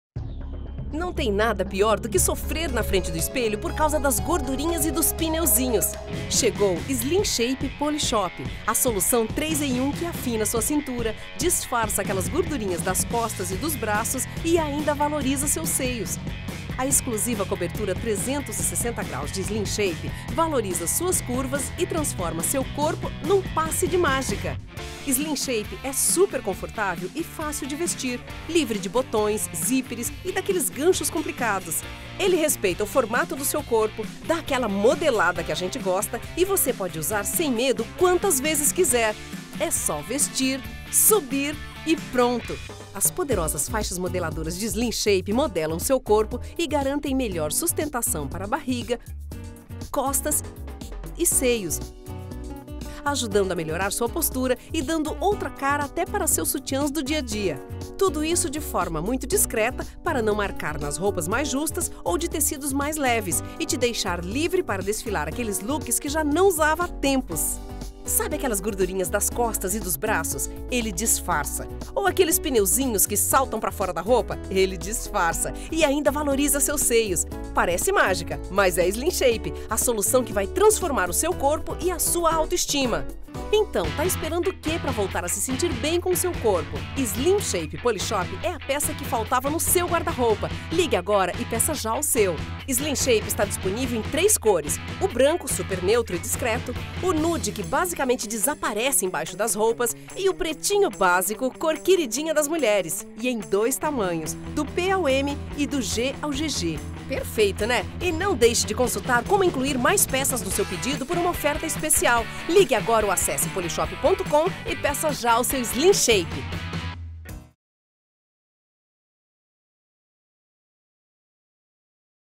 Slim Shape | Polishop
Modela a cintura, valoriza o busto e ajuda a melhorar sua postura! ou ligue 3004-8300 (custo de ligação local).

Slim Shape Polishop é a inovação que modela seu corpo na hora!* Ele afina sua cintura, disfarça as gordurinhas do abdômen e das costas e ainda valoriza seu busto para você ganhar uma silhueta elegante!

Tudo isso graças à sua exclusiva compressão graduada, que oferece a intensidade certa para modelar cintura, abdômen e costas, enquanto ressalta suas curvas e ainda ajuda a melhorar sua postura!

Desenvolvido em material leve e elástico, Slim Shape é superconfortável e se adapta ao seu corpo. E graças ao seu design sem costuras, zíperes ou fechos, você vai contar com total facilidade na hora de vestir!

Slim Shape é ideal para ser usado em todos os momentos do seu dia. Superdiscreto, ele não enrola, não incomoda e nem marca sob a roupa. Demais!

Slim Shape está disponível em 2 tamanhos que vestem do P ao M e do G ao GG, ideal para diferentes tipos de corpo!

Slim Shape é uma superexclusividade Polishop!